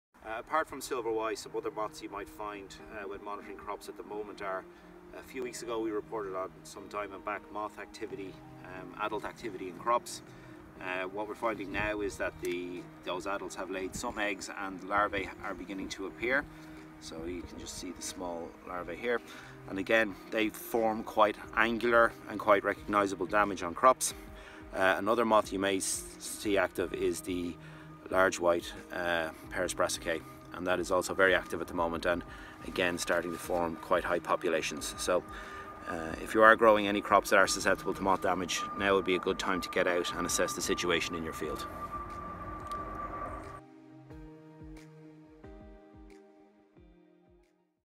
General moth types on Brassicas
Michael Gaffney describes some more moths to watch out for on brassicas.